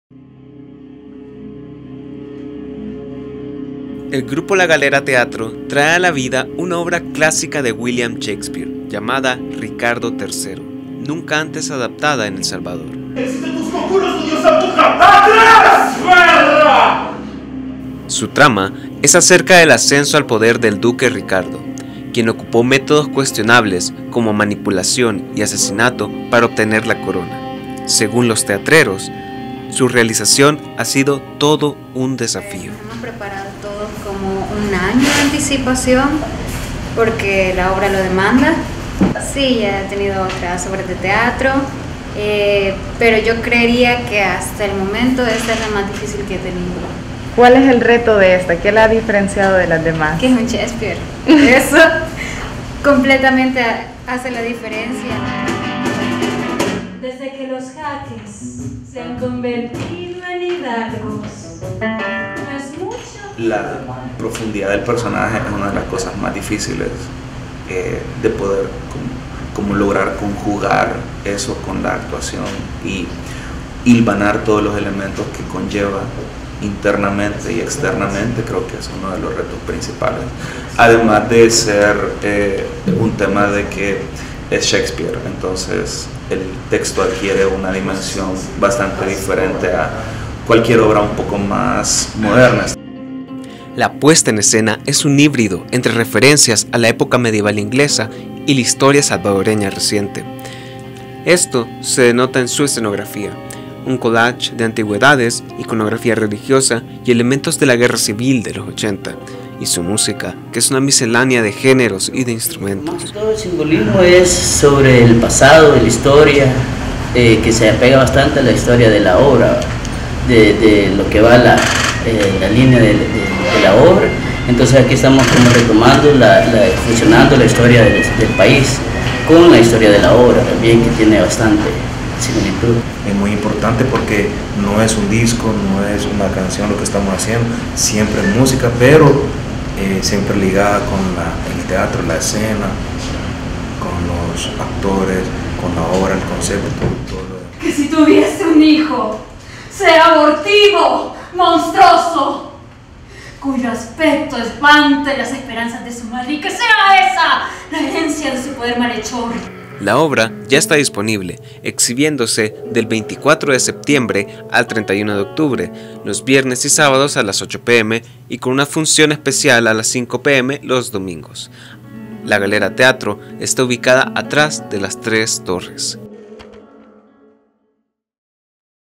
Galera Teatro trae a la vida una obra de Shakespeare
El grupo La Galera Teatro trae a la vida una obra clásica de William Shakespeare, llamada Ricardo III, nunca antes adaptada en El Salvador. Su trama es acerca del ascenso al poder del Rey Ricardo de Inglaterra, quien ocupó métodos cuestionables como asesinato y manipulación para obtener la corona.

¡Visítenos!